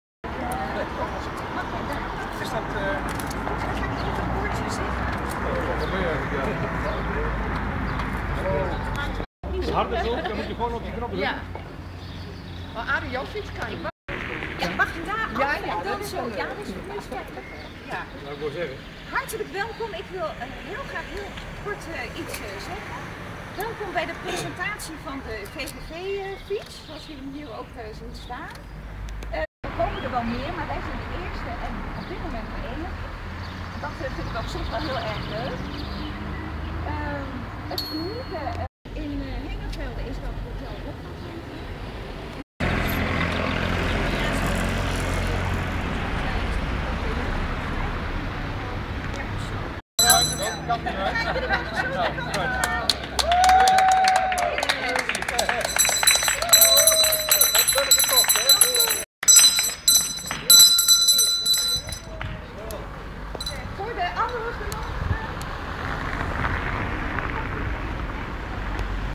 VVV verhuurt electrische fietsen
De VVV Nederland gaat met de tijd mee. De electrische fiets neemt een enorme vlucht en daar wil de toeristische vereniging graag op in springen. De VVV Hof van Twente is nu nog de enige in Twente die meedoet. Vanmorgen was de aftrap bij de pomp op de Markt in Delden.